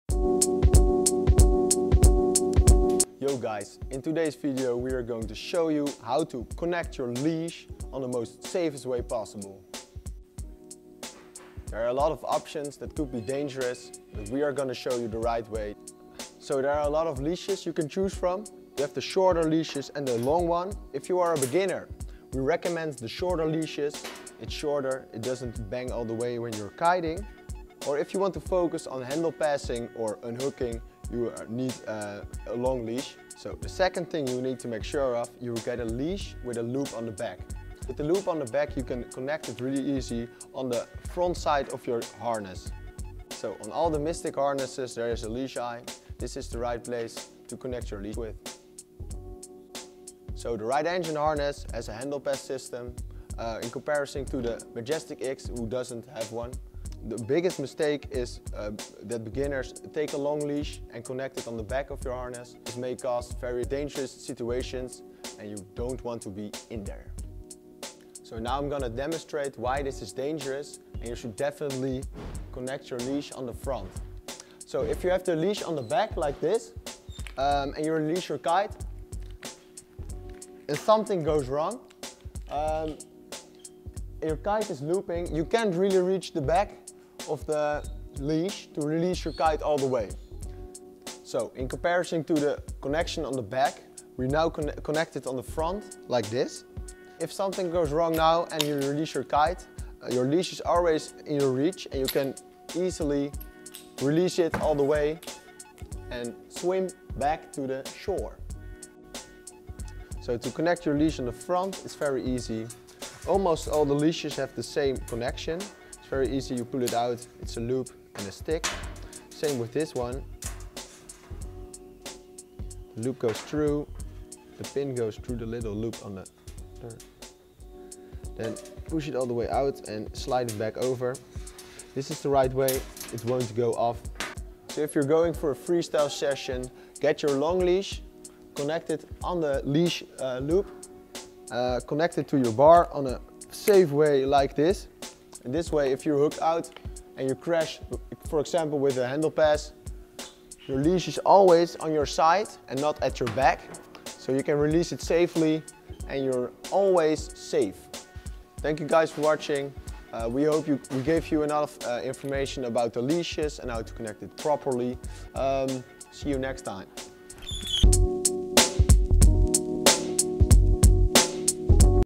HOW TO: Connect your leash the right and safest way
In this video we show you how to connect your leash on the right and safest way possible!